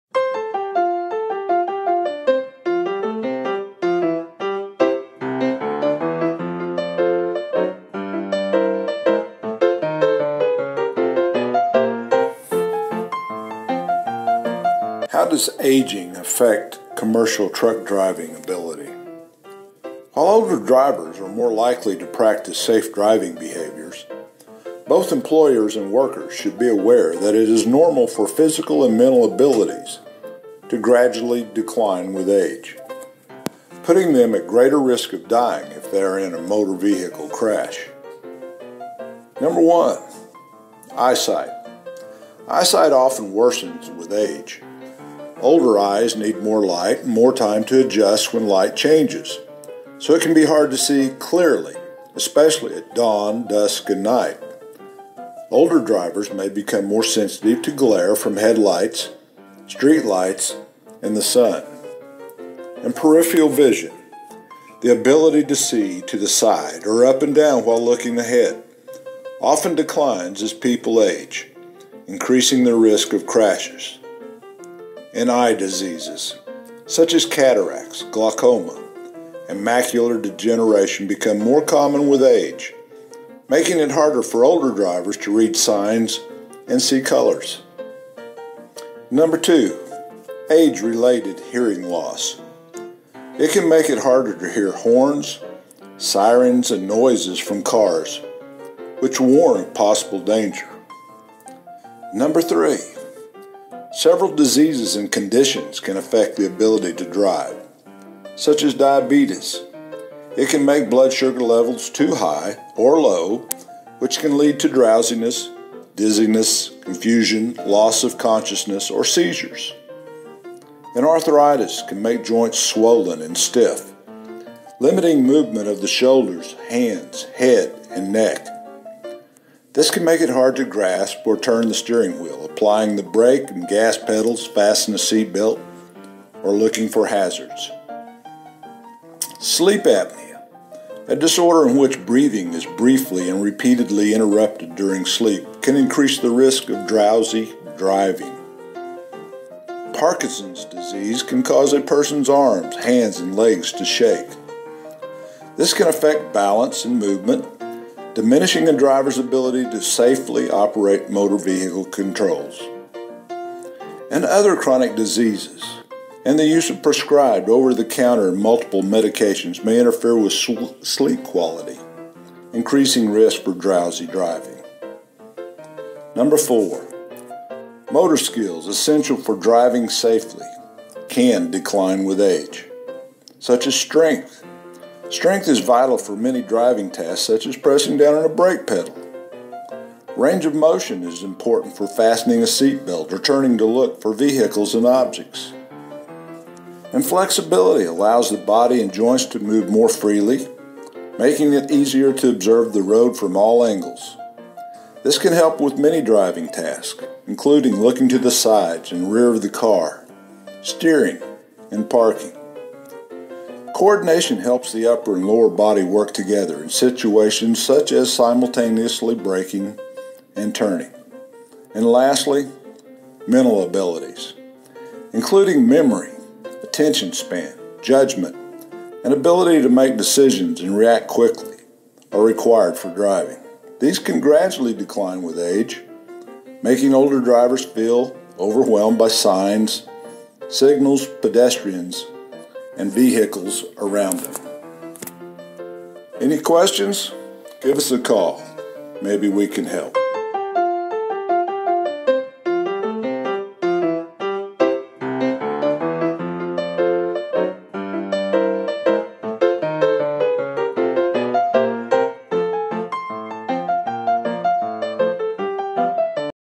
"How Does Aging Affect Commercial Trucking, Driving Abilities & Insurance??"
While older drivers are more likely to practice safe driving behaviors, both employers and workers should be aware that it is normal for physical and mental abilities to gradually decline with age--putting them at greater risk of dying if they are in a motor vehicle crash...Every company in Oklahoma that has an employee must carry workers compensation.  Competitive pricing is available through the Oak Tree Agency in Edmond, Oklahoma.